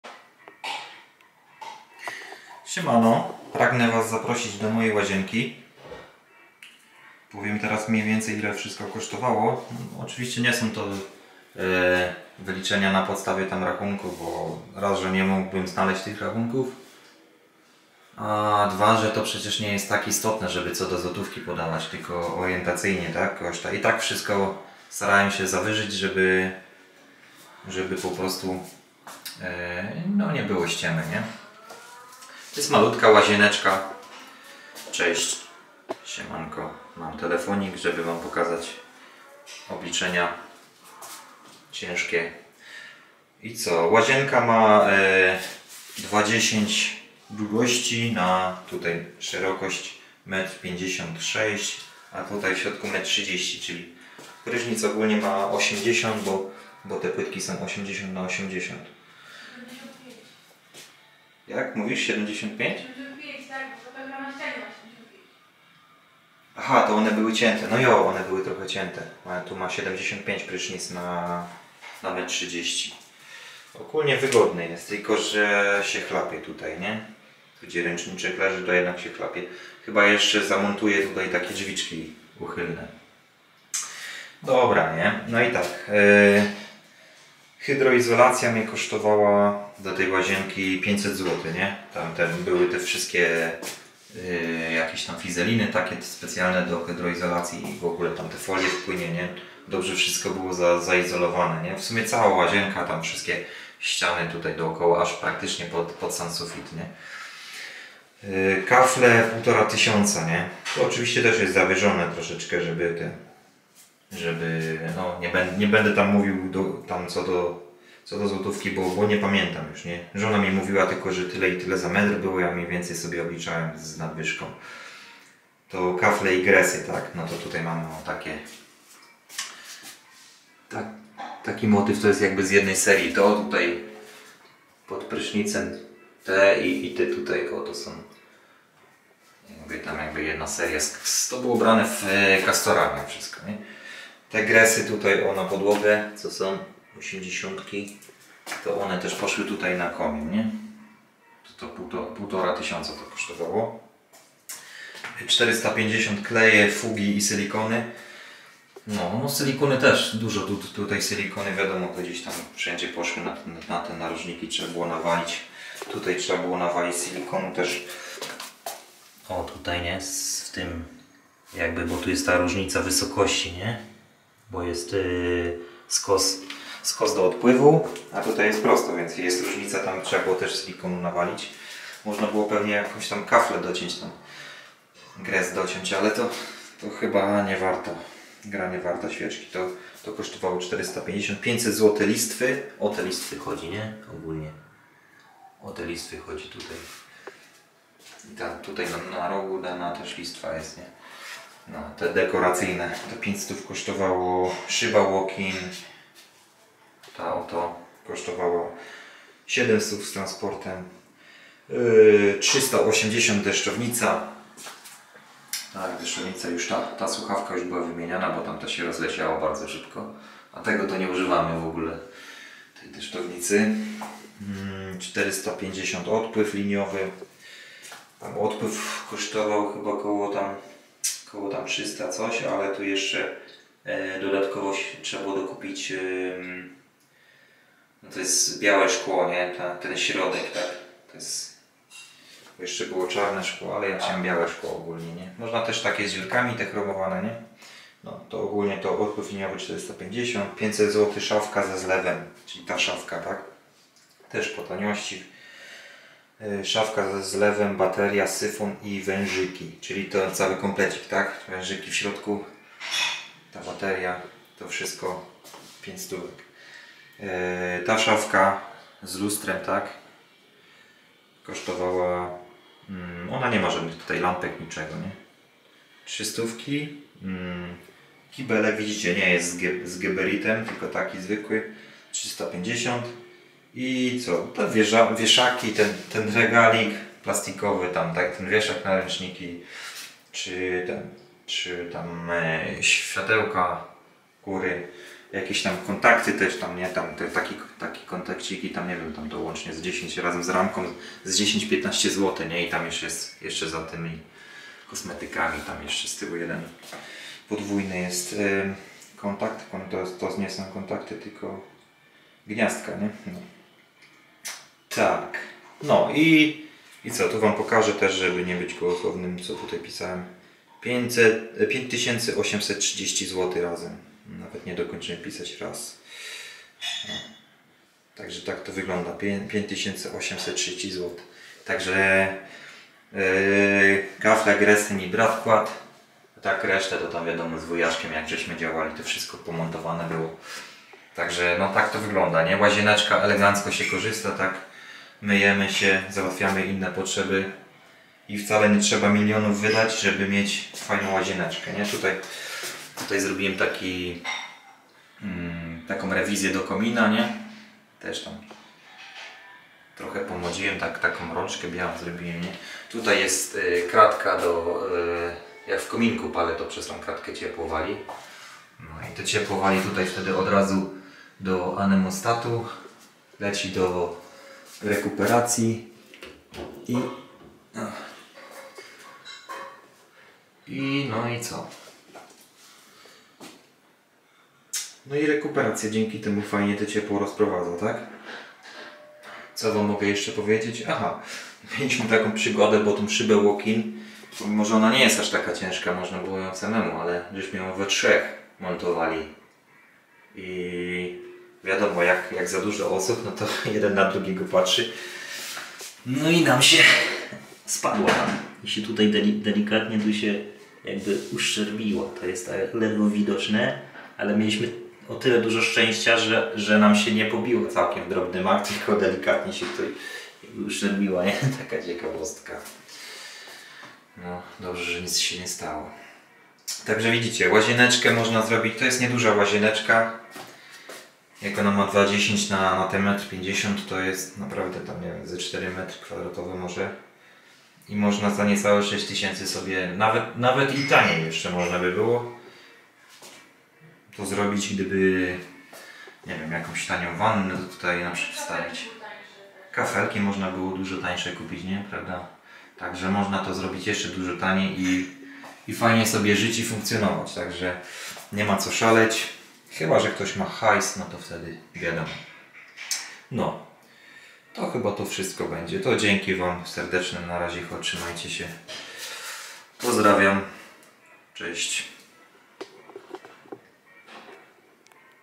Ile kosztuje łazienka 3m2. Podsumowanie kosztów. Koszt wykończenia małej łazienki.
Siemanko
Łazienka za 6 tys. zł
Podsumowanie kosztów z 2022 r.